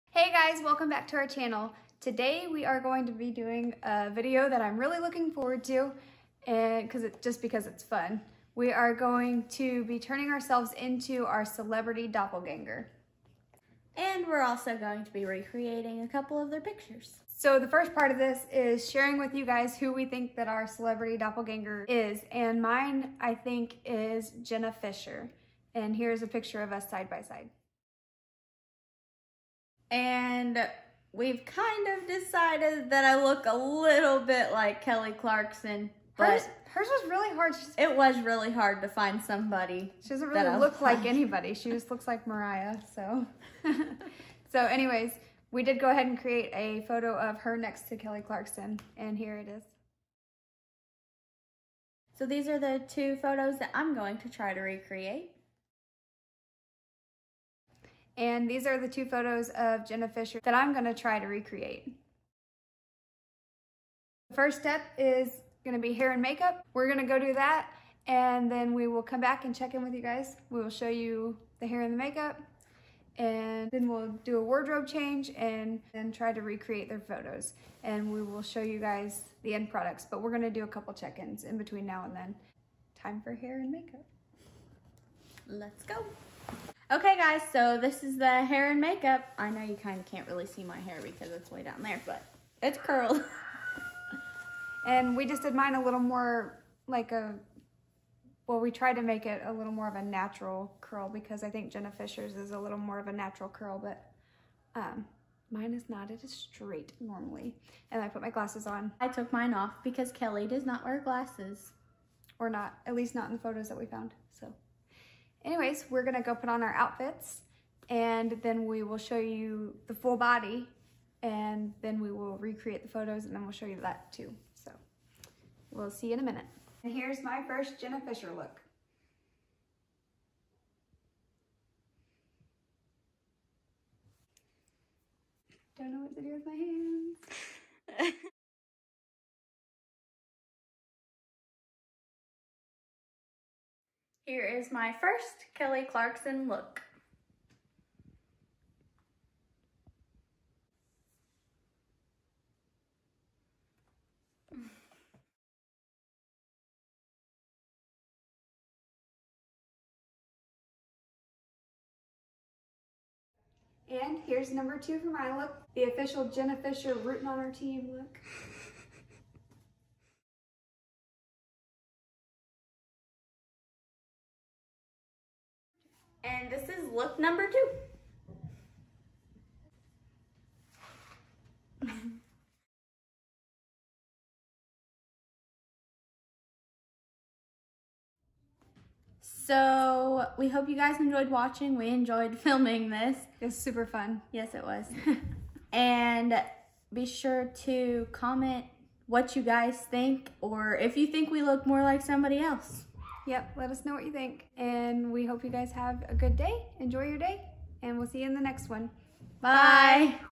Turning Into Our Celebrity Doppelgänger | adustydiamond
In this video we did some research and tried to find our celebrity doppelgänger. Then we tried recreating two of their pictures each. This was super fun to make! Let us know if you think we look like them...enjoy!